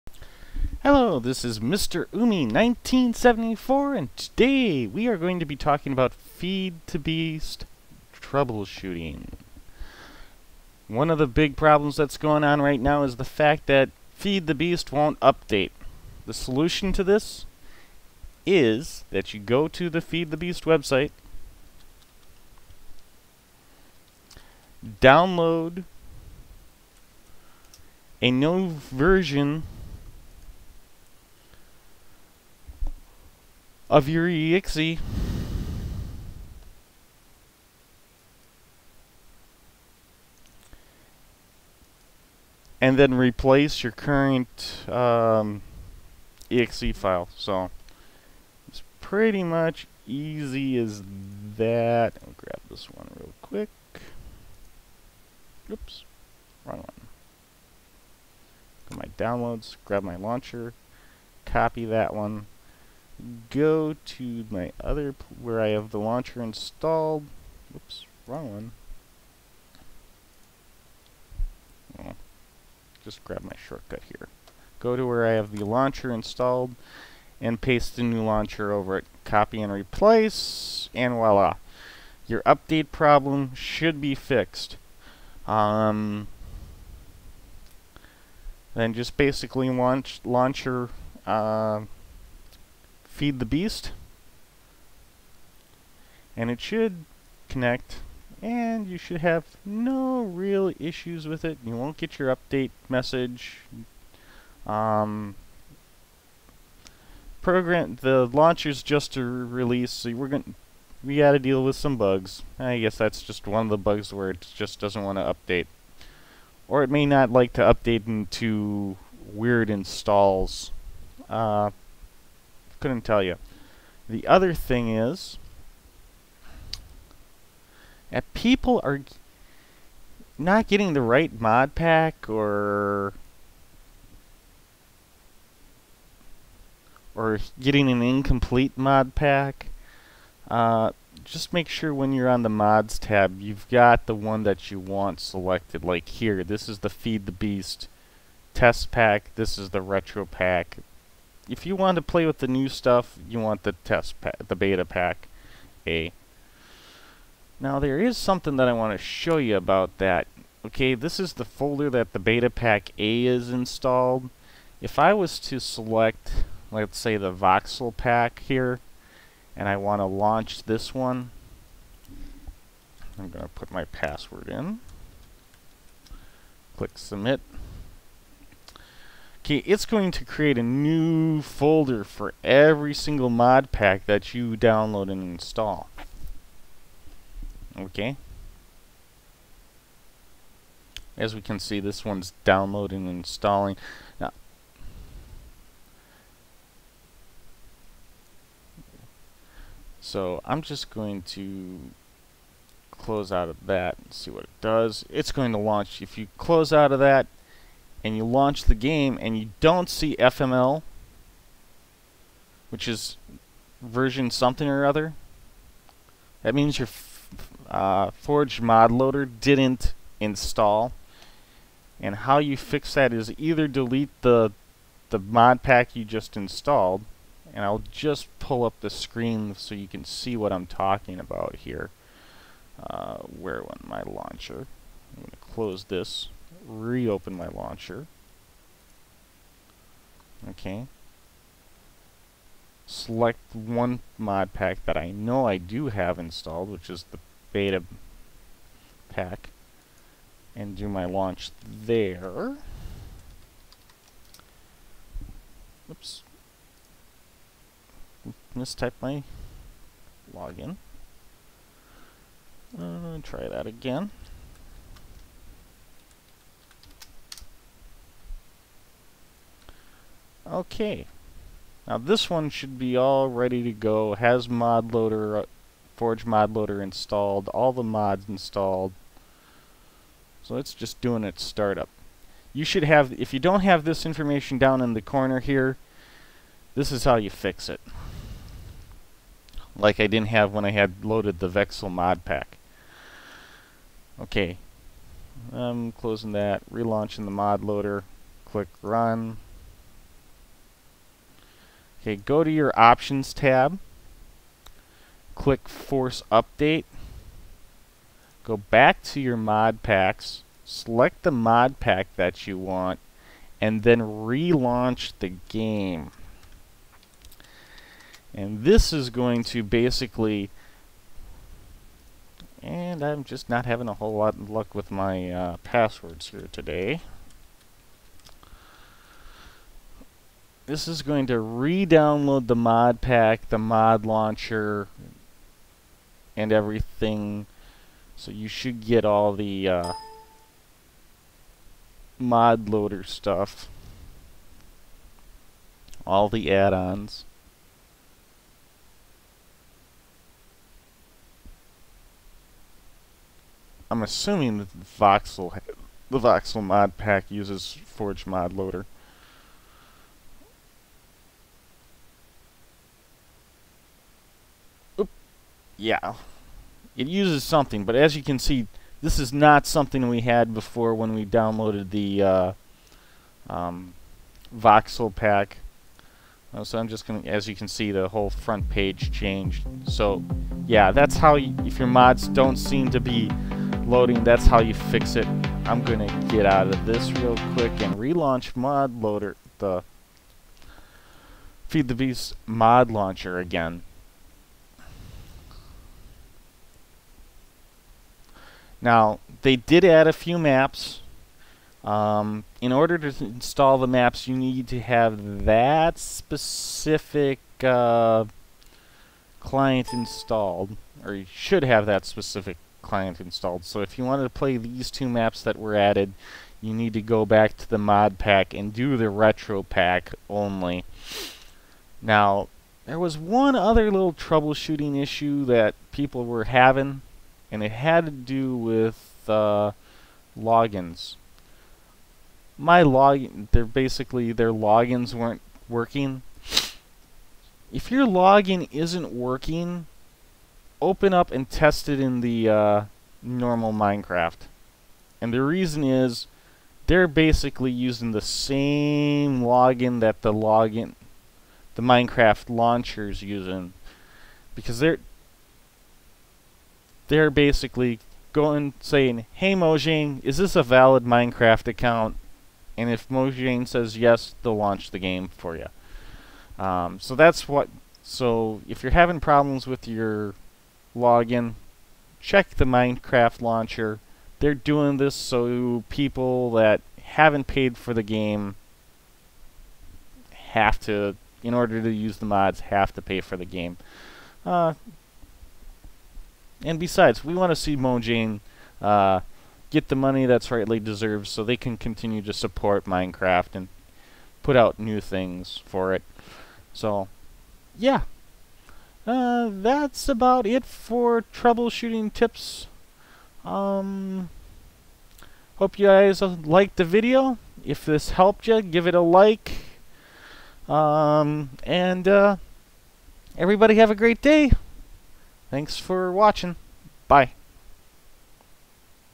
Feed the Beast Mod Loader - Trouble Shooting
Easy fixes for Feed the Beast problems
- Updater will not install
- Missing mods or FML
- login issues